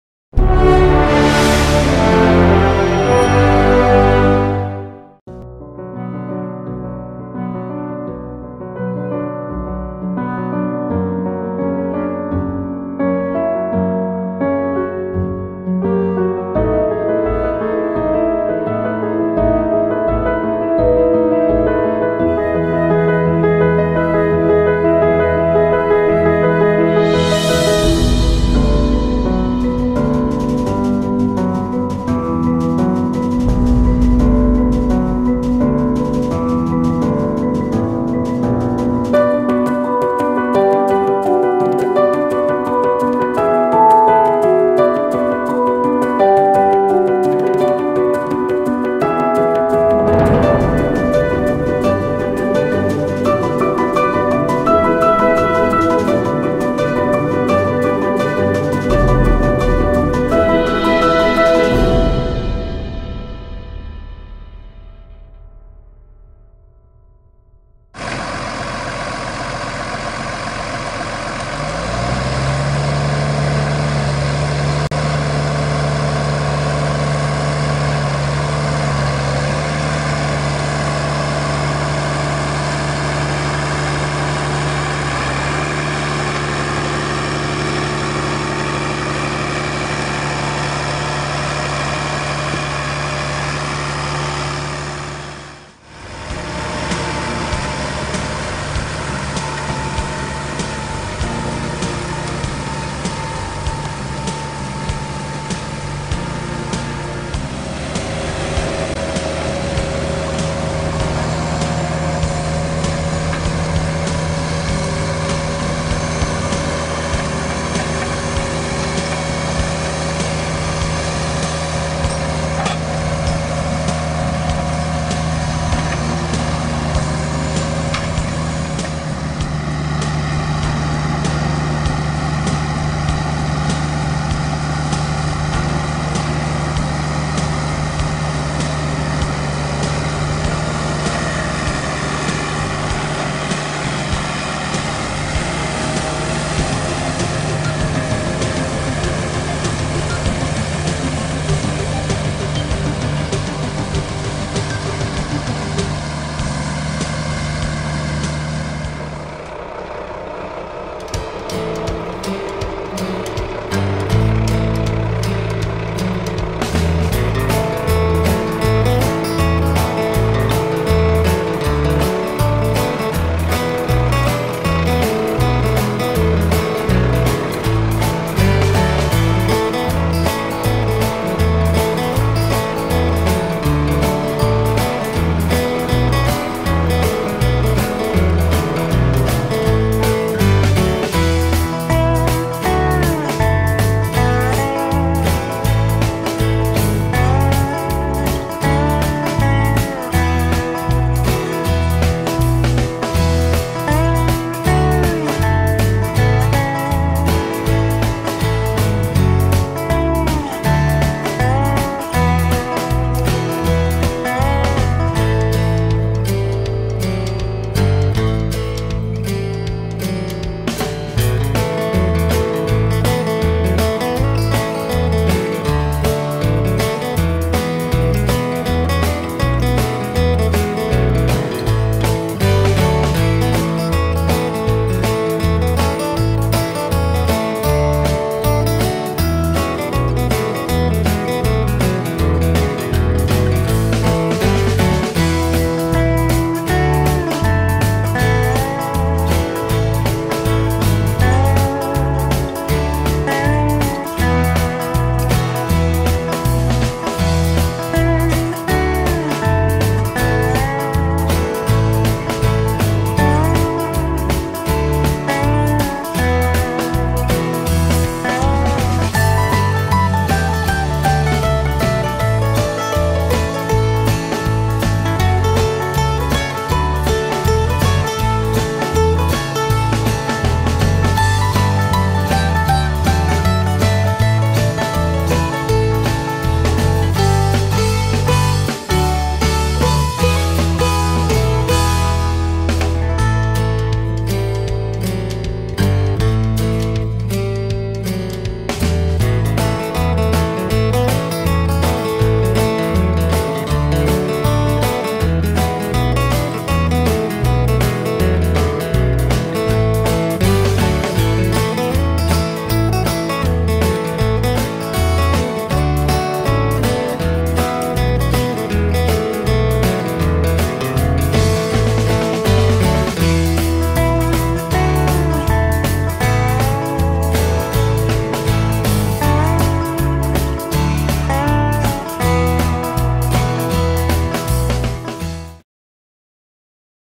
Aerway Manure
This is the AerWay unit working to incorporate manure with draglines, SSD technology and standard quick adjust models